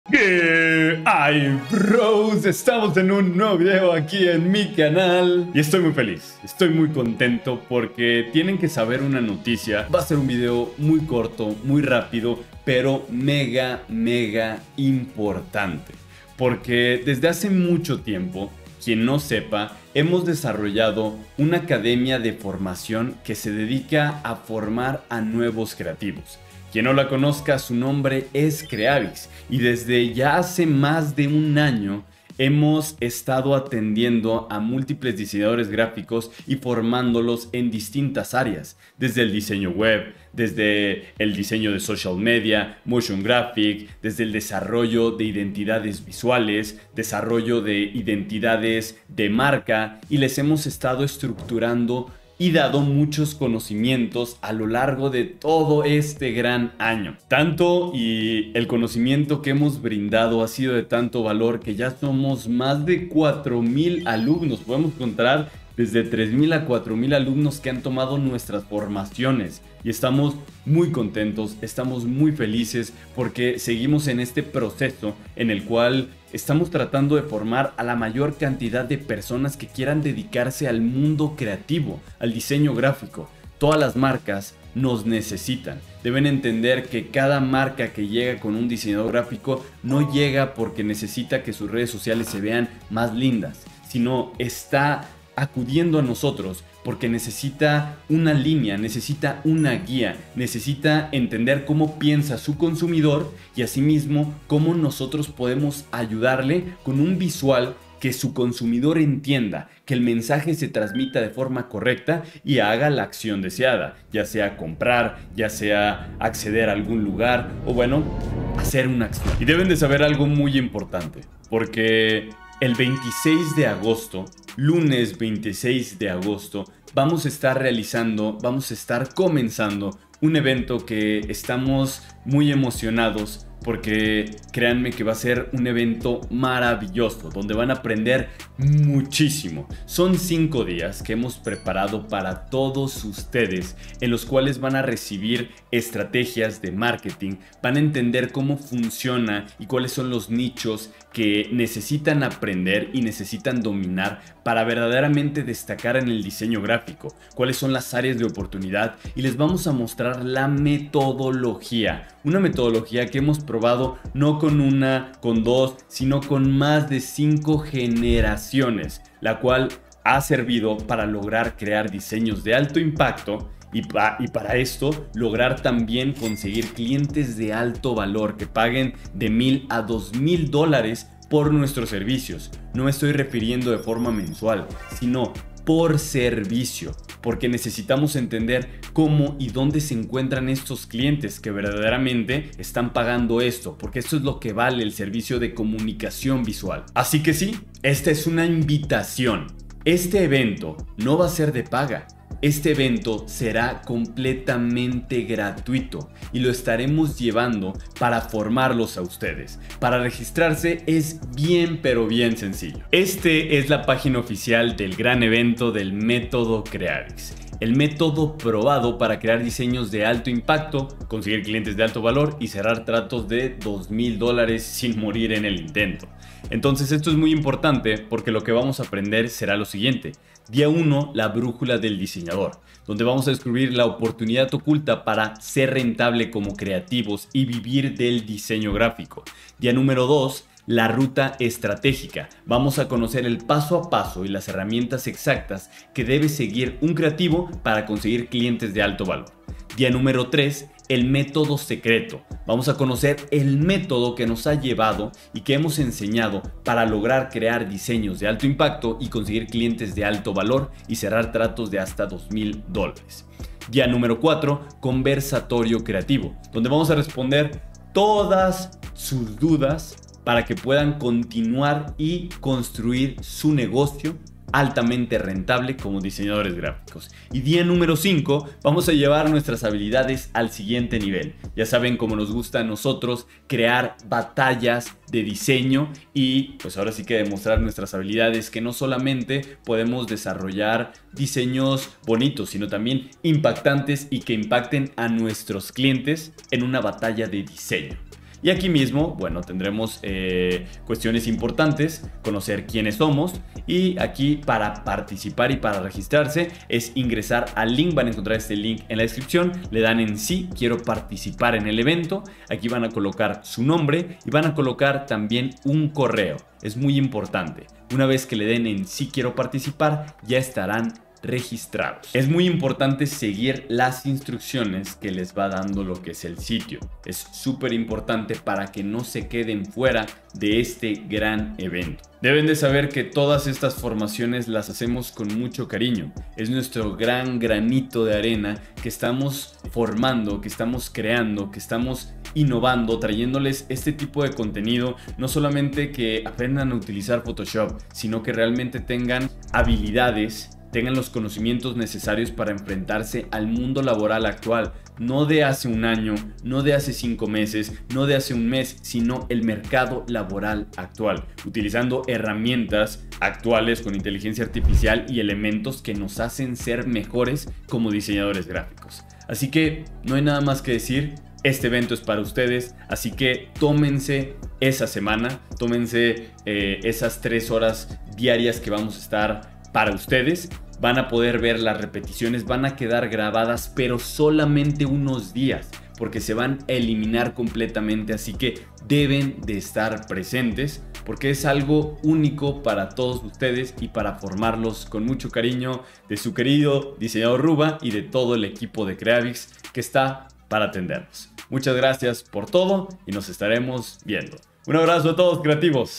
Conviértete en un DISEÑADOR EXPERTO y vende PROYECTOS de +1000 dólares!!
DESCUBRE la Guía, los pasos, LA BRÚJULA y conoce el juego que debe jugar un creativo para ser rentable y vivir del diseño gráfico sin morir en el intento.

🚨 Para quién NO es el MÉTODO KREAVIX:

Personas que desean dinero rápido.
Personas que no les gusta invertir en su formación.
Personas que quieren todo facil con un solo video de Youtube.

¡Prepárate para transformar tu enfoque en las ventas digitales y llevar tu engocio como creativo al siguiente nivel!

🗓️ Fecha y Hora: Lunes 21 de Agosto | 5:00 P.M. (Hora México)